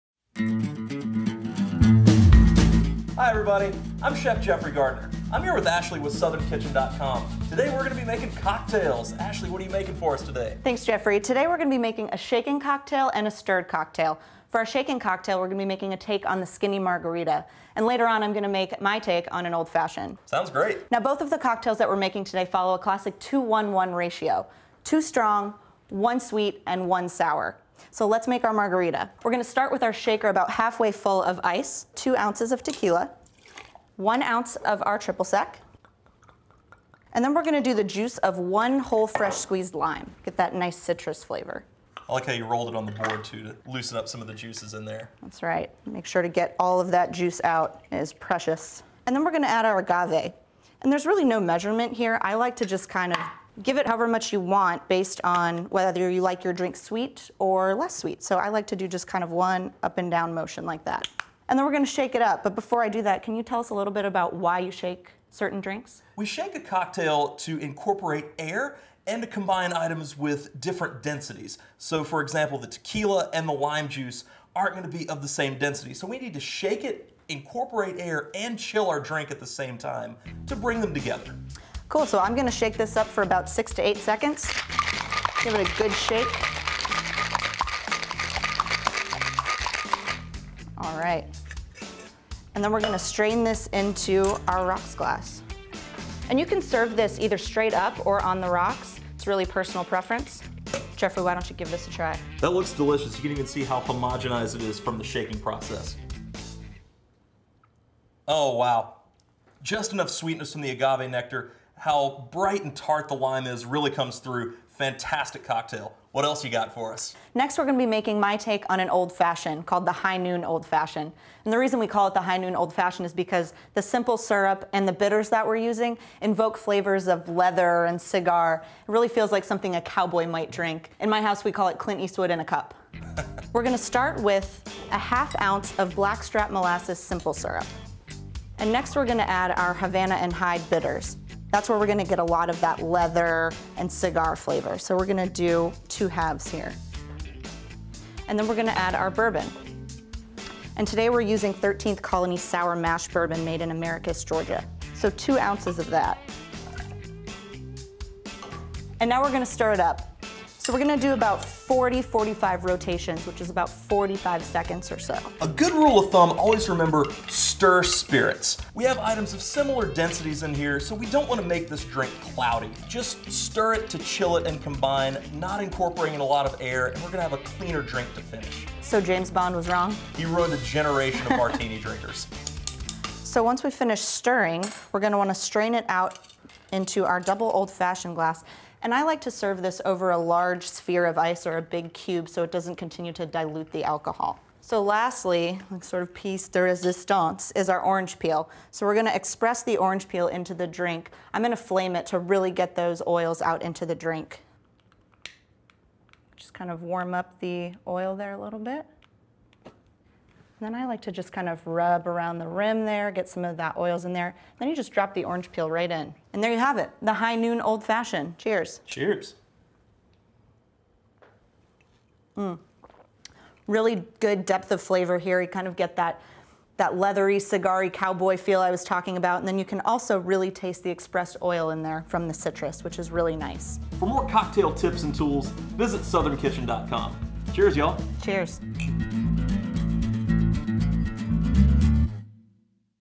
How To Craft Cocktails: A Skinny Margarita & An Old Fashioned
Learn how to mix up and craft cocktails with our founding Editor-In-Chief and Chef Jeffrey Gardner! You'll want to learn to how to make a shaken skinny margarita and a stirred old fashioned.